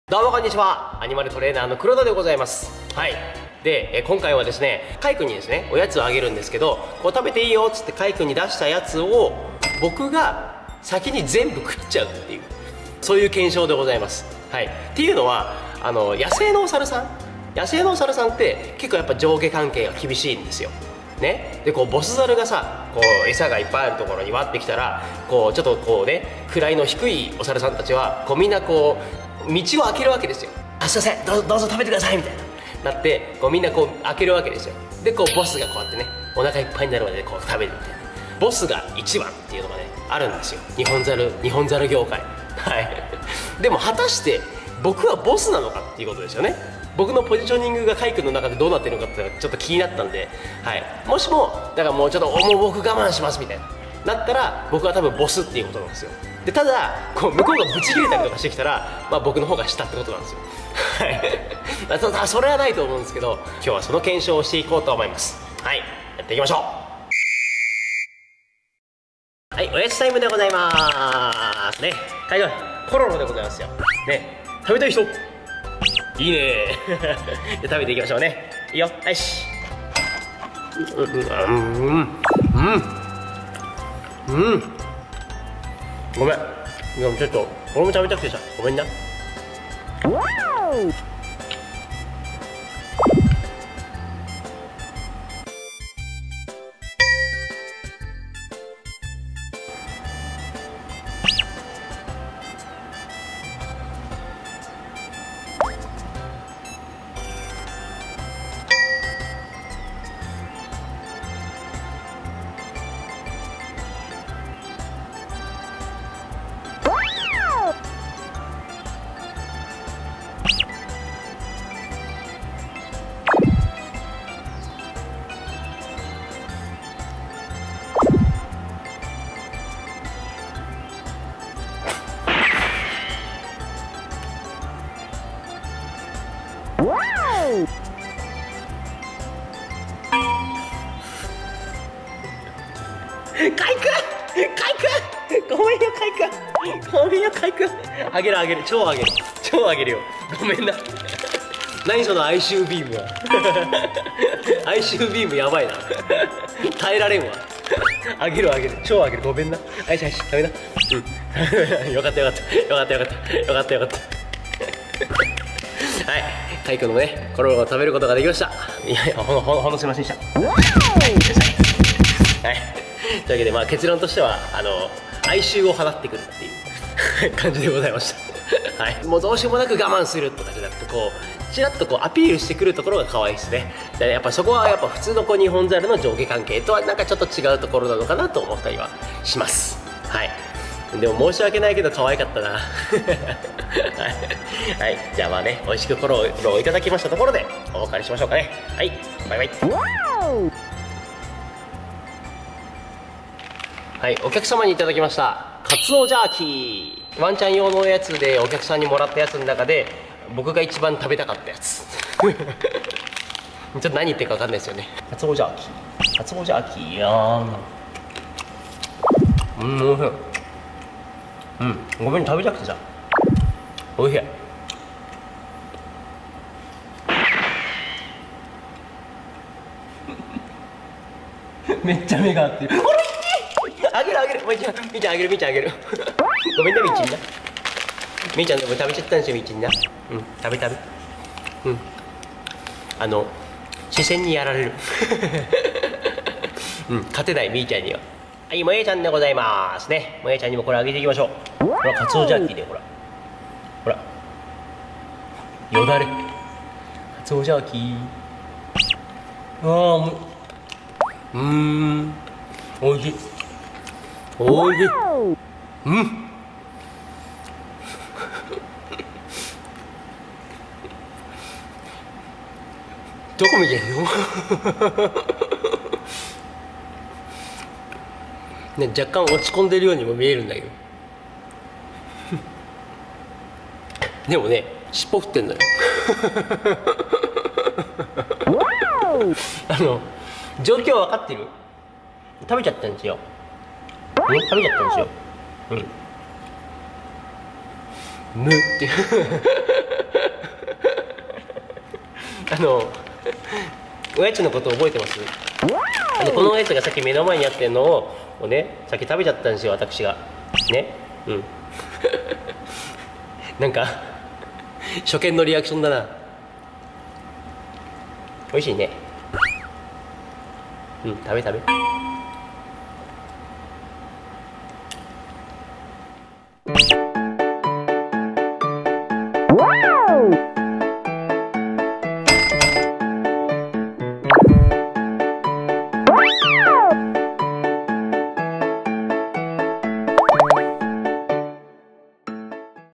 お猿さんのオヤツを飼い主が全部食べちゃったら…どうする？
ニホンザルの雄は縦社会ですからねぇ。。それがどう出るか…！？！？

おサルさんとこんなことしてほしいっていうリクエストもお待ちしております！